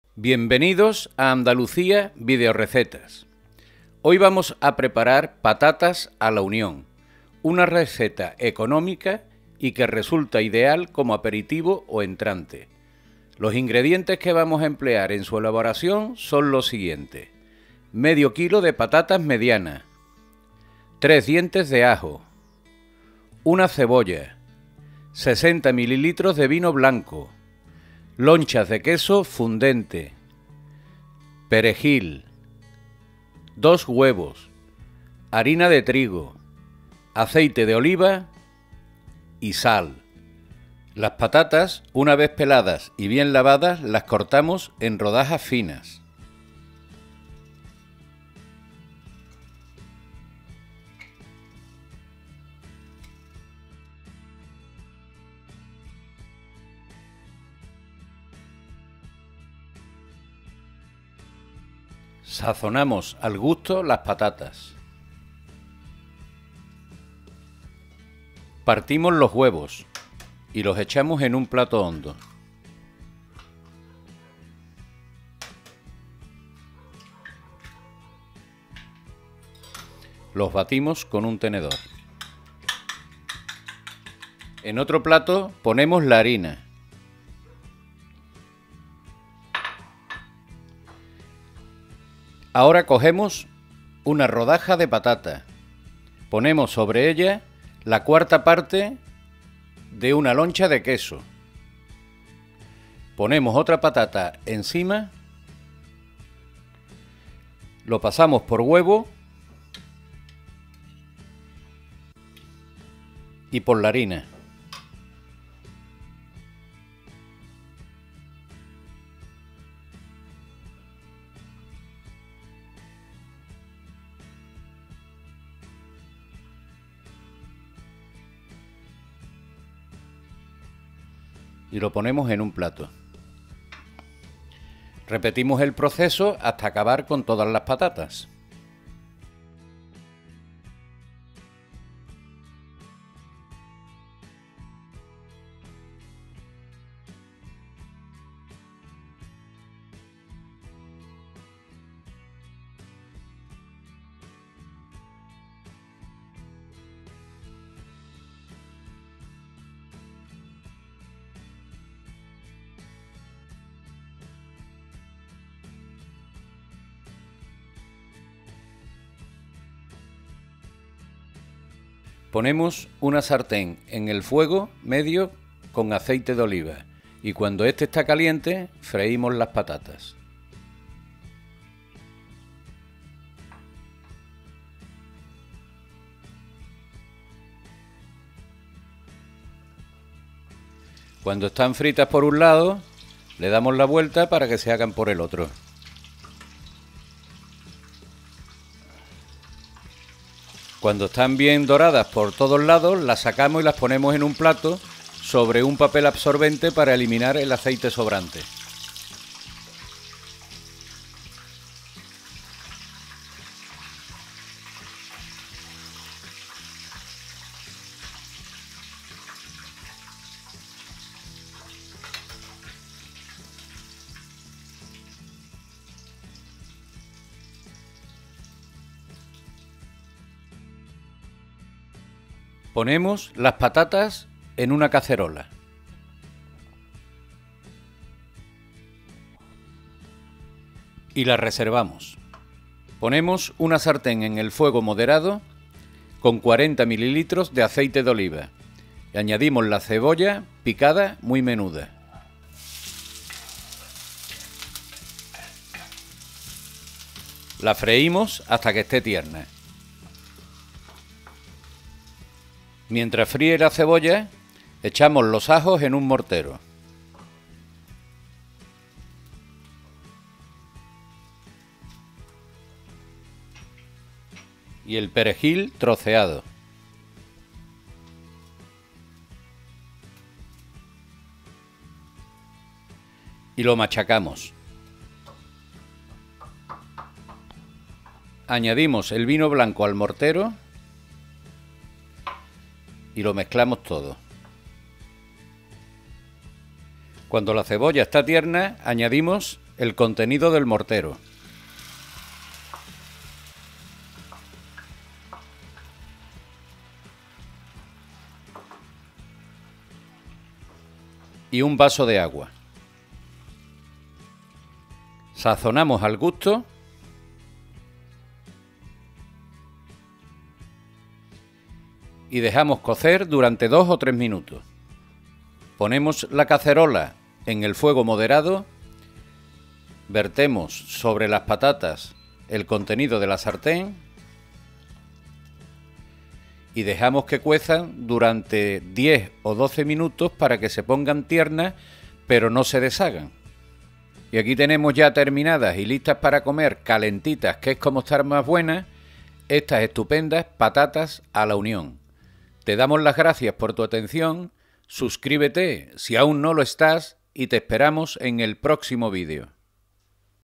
Patatas a la unión
Receta de cocina sobre como hacer patatas a la unión. Desde ingredientes y cantidades, hasta como elaborar unas deliciosas patatas a la unión.

INGREDIENTES:

½ kilo de patatas medianas
3 dientes de ajo
1 cebolla
60 ml de vino blanco
Lonchas de queso fundente
Perejil
2 huevos
Sal